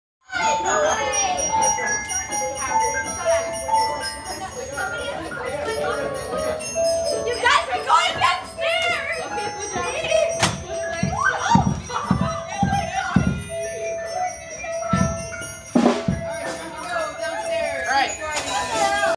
Raymond a Colorguard?
Raymond practicing with a rifle.